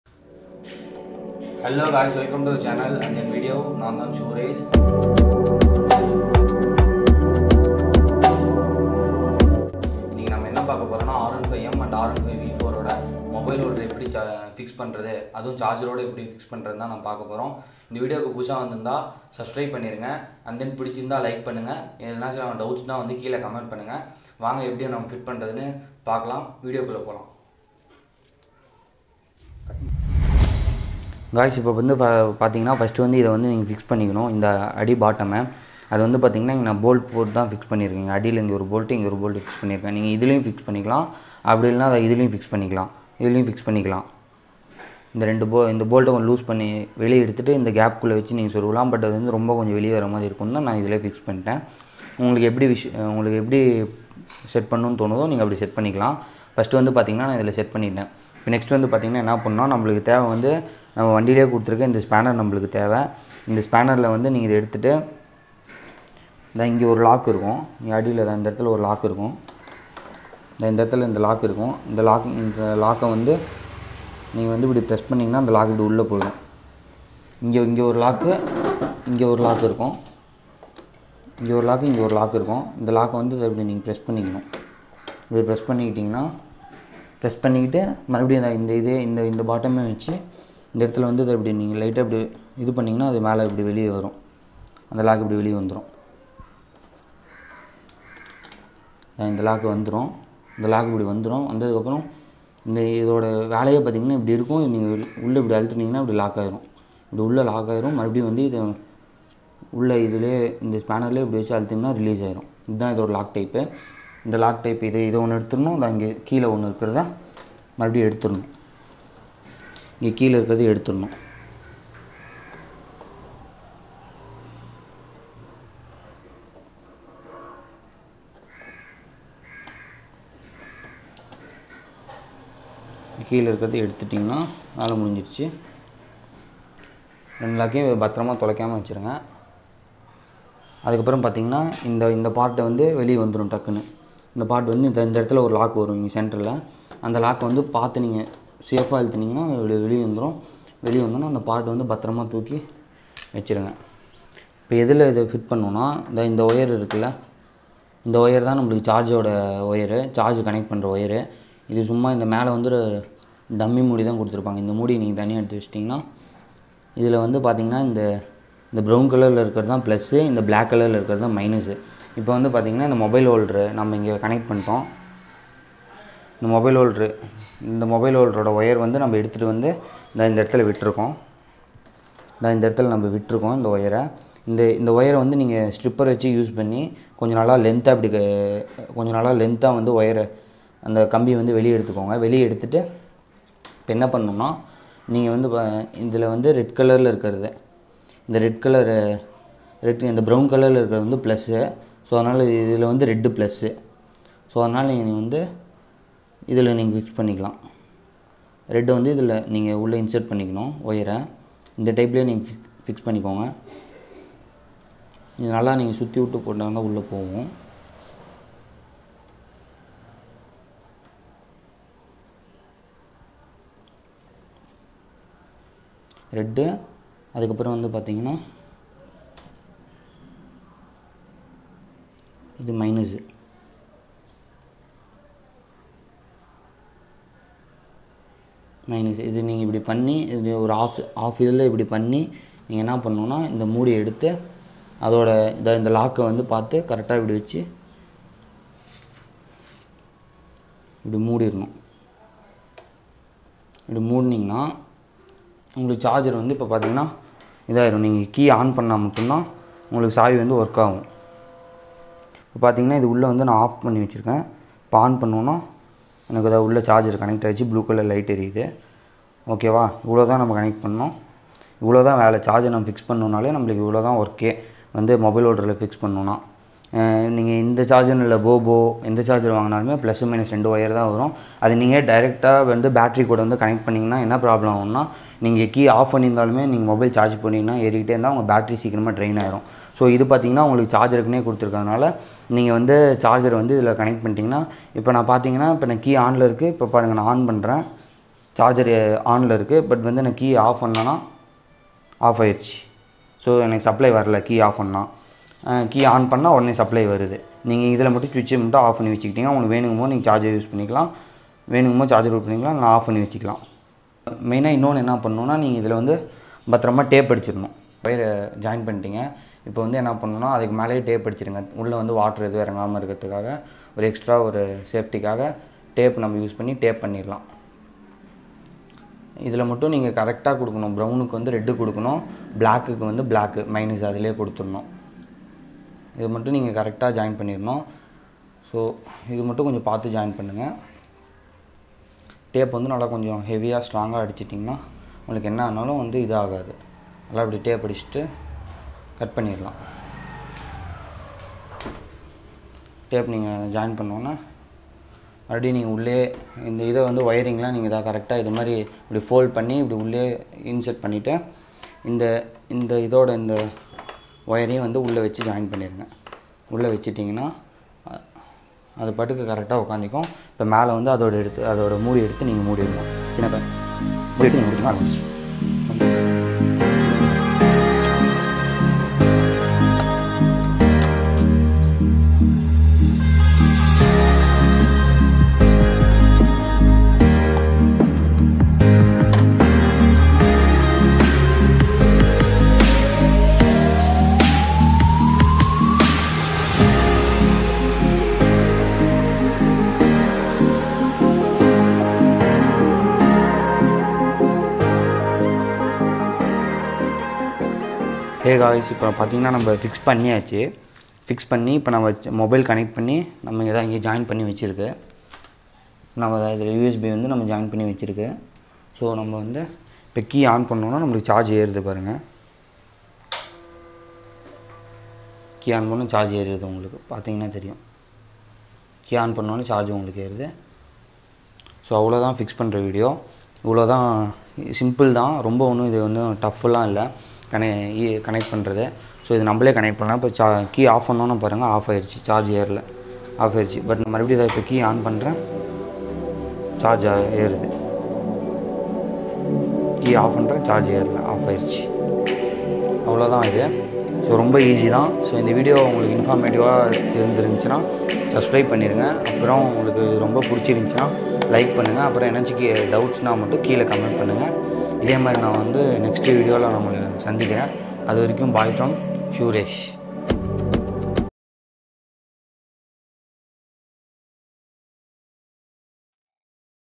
R15 v4 m mobile holder fixing with charger ⚡ | in tamil | shoe race
how to fix mobile holder with charger fixing in r15 v4, r15 m, r15v3, r15s in tamil
easy way and simple steps to fix a charger